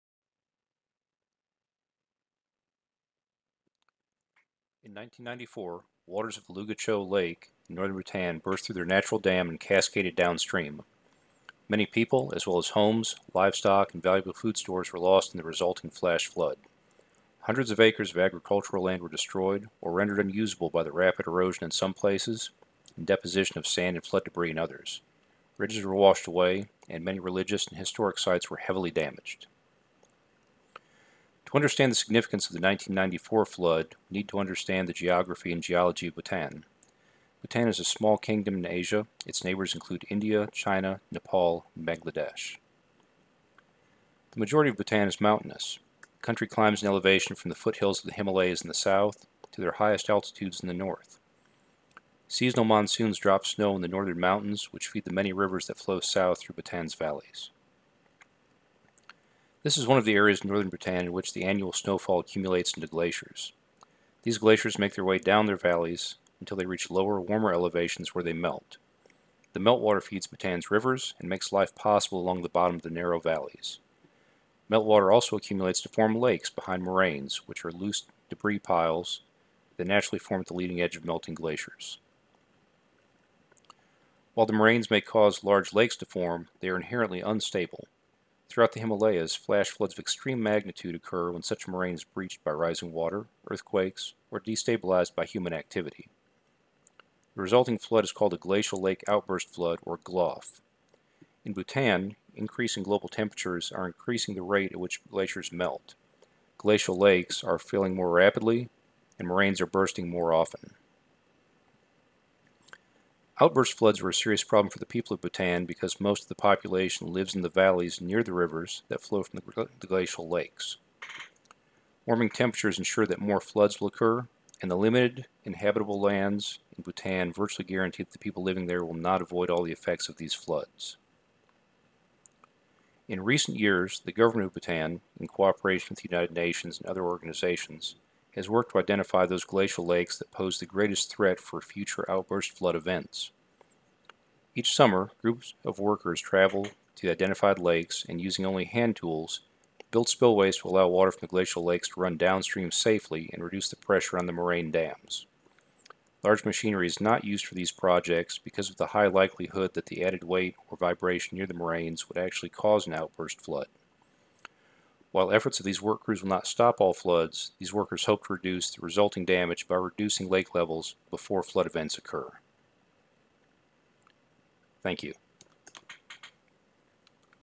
Bhutan Glacial Lake Outburst Flood 1994
By Chris Norcross.  This is a video project for NC State University's MEAS 100 Earth System Science class.

In October 1994, a Glacial Lake Outburst Flood (GLOF) occurred in the Himalayan country of Bhutan, causing significant damages and loss of life.  GLOF events are particularly hazardous in Bhutan because so many of Bhutan's people live in the bottoms of the narrow valleys that run down from the high mountains.  Increasing global temperatures have increased the rate of glacial melting in Bhutan and elsewhere, increasing the frequency and severity of GLOF events.